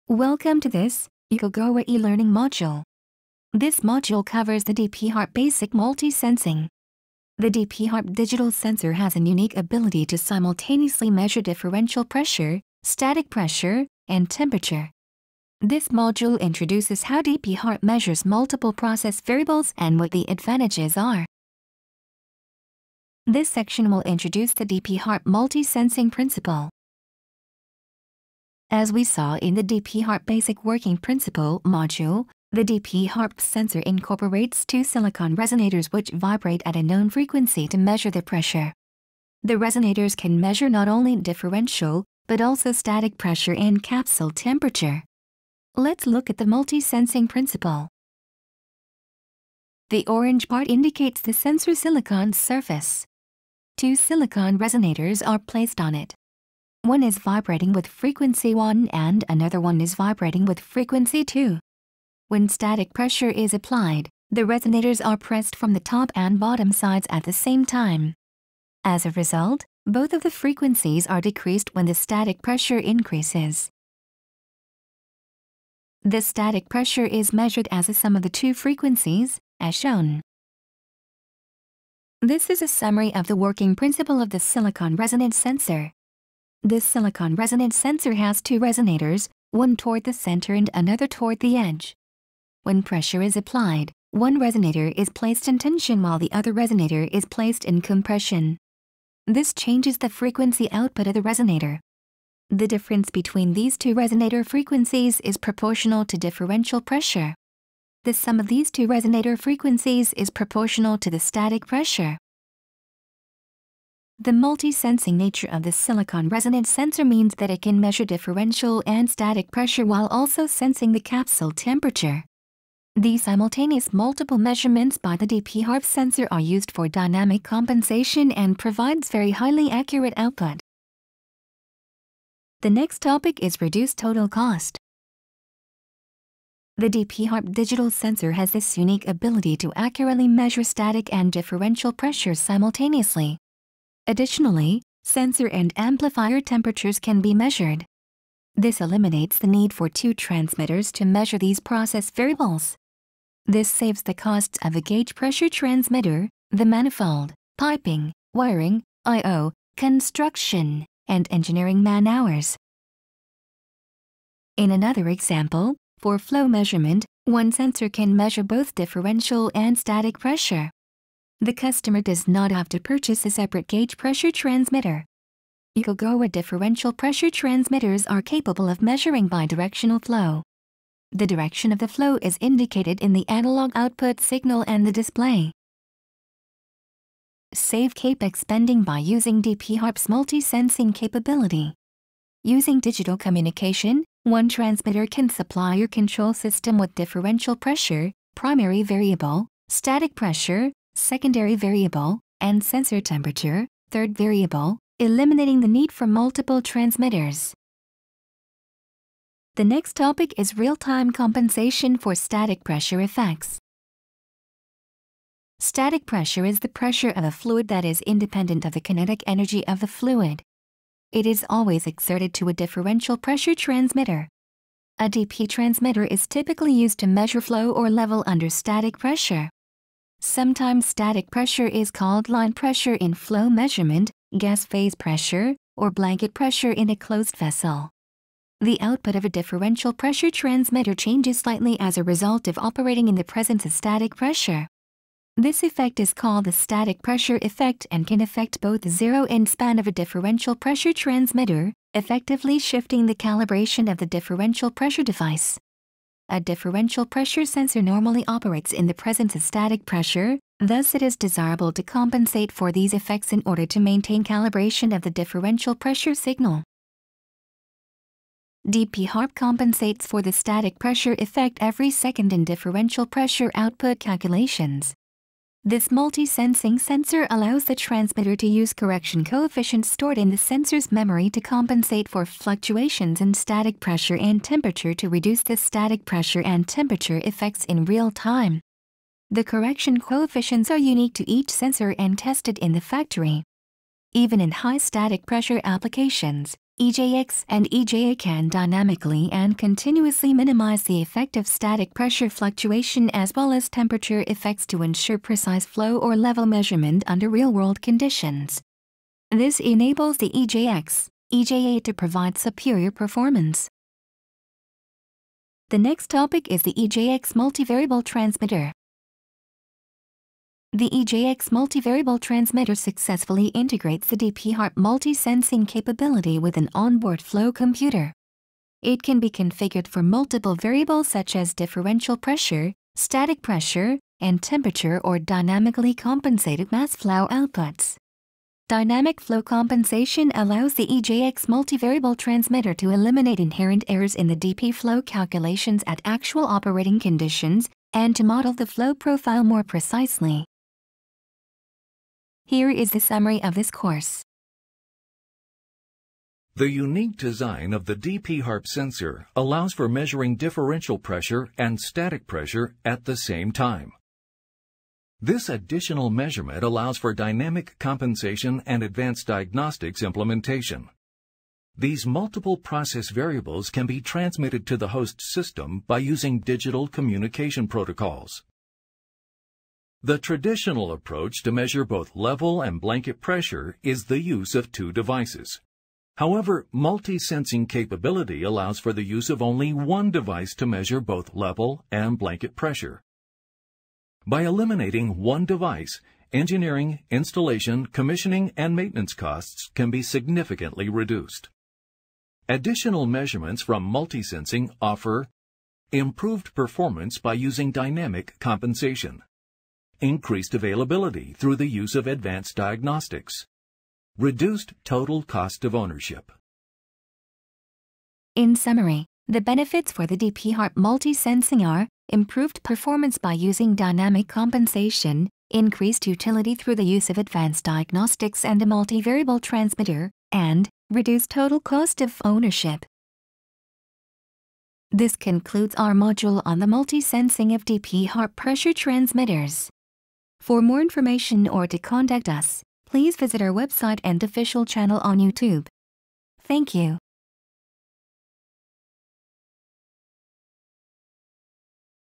4. Pressure Transmitter (DPharp Series) - Multi-sensing -
This Yokogawa e-learning module covers the multi-sensing for DPharp pressure transmitters. You will learn about the multi-sensing principle, its superior performance, and how total cost is reduced.